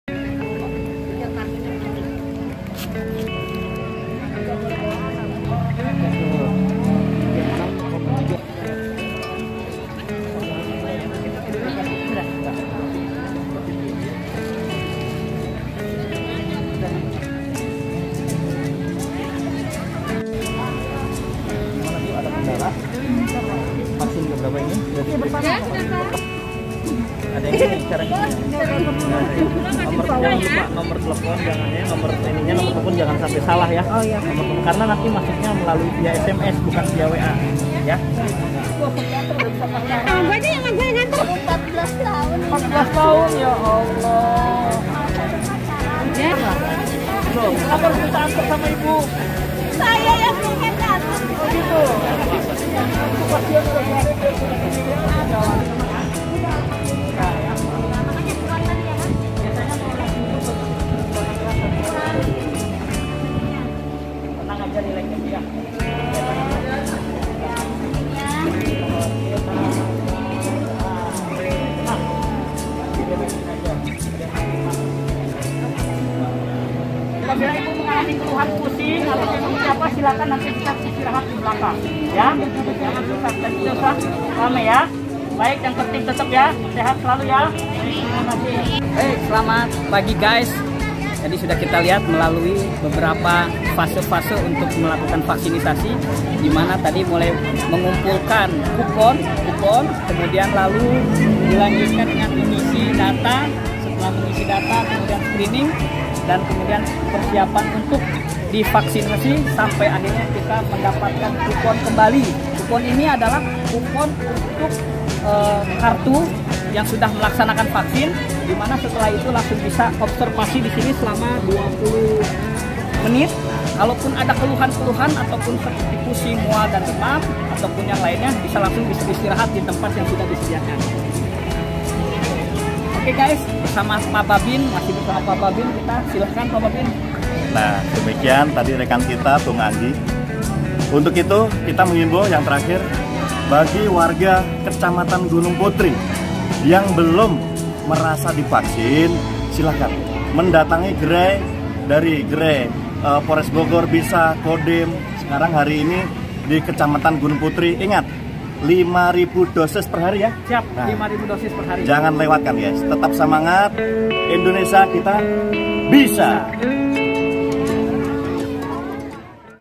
Serbuan Vaksin
Giat Vaksinasi hari ke 2 di kantor Kecamatan Gunung Putri yang menjadi Sentra Vaksinasi 5000 vaksin 1 hari.